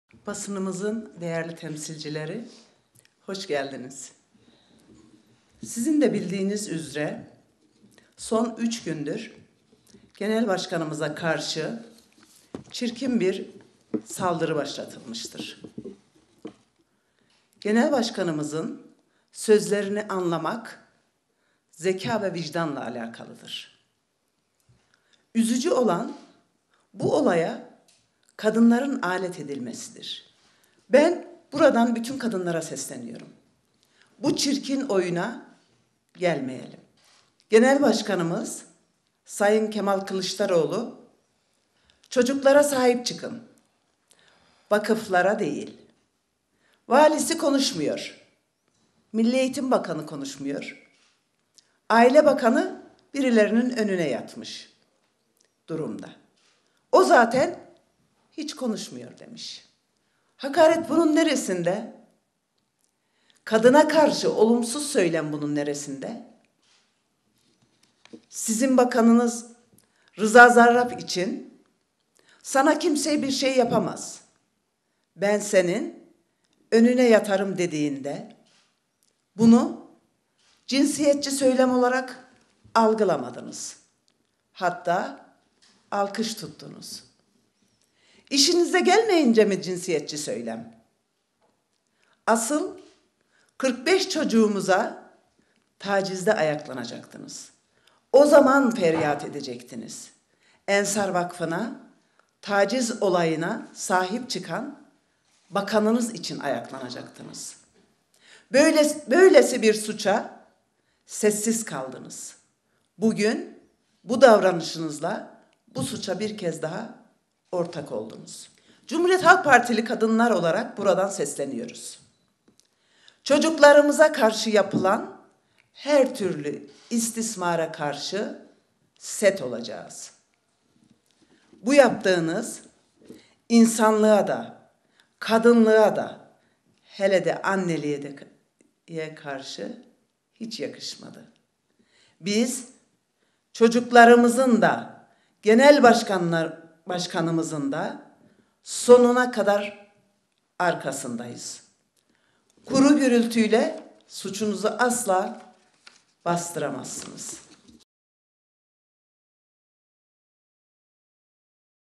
Bu Çirkin Oyuna Gelmeyin!
Kadın Kolları Başkanı Fatma Köse: Eski İçişleri Bakanı Reza Zarrab için bu ifadeyi kullanırken cinsiyetçi algılamadınız da, Genel Başkanımız kullanınca mı cinsiyetçi oldu bu ifade? Asıl, 45 çocuğumuza tecavüz edilirken ayaklanacaktınız, feryat edecektiniz, Aile Bakanı’na tepki gösterecektiniz!